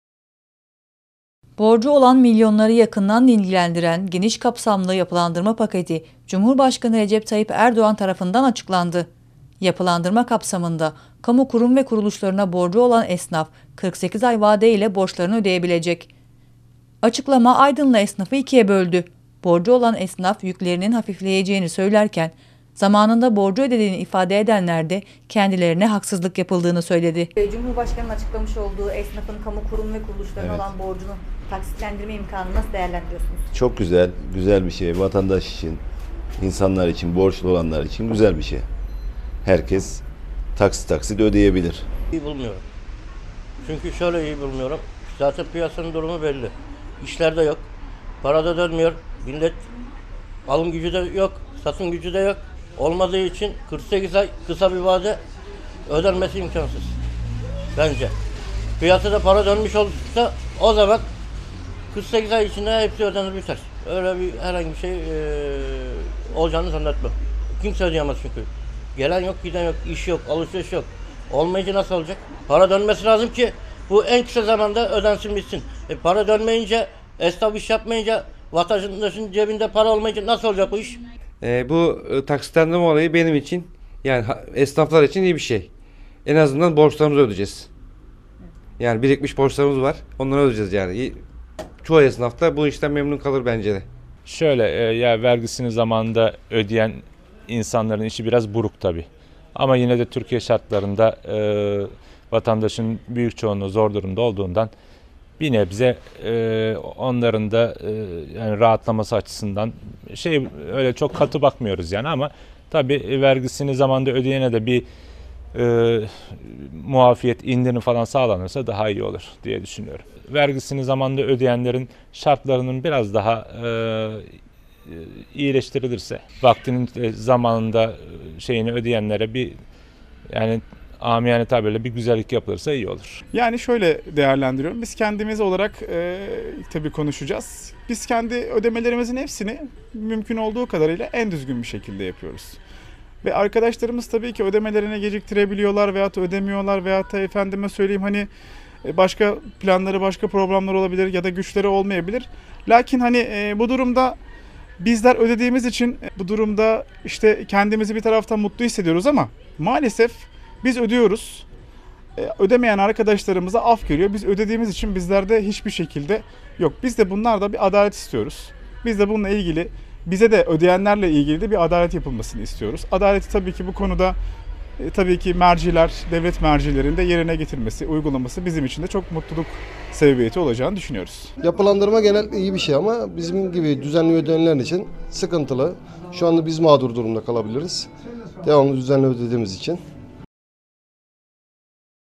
Yapılandırma Aydınlı esnafı ikiye böldü
Cumhurbaşkanı'nın açıkladığı kamu kurum ve kuruluşlarına olan borçların 48 ay taksitle yeniden yapılandırılacak olmasını Aydınlı esnafa sorduk. Yapılandırmadan hoşnut olanlar kadar, zamanında borçlarını ödedikleri için kendilerine haksızlık yapıldığını düşünenlerde var.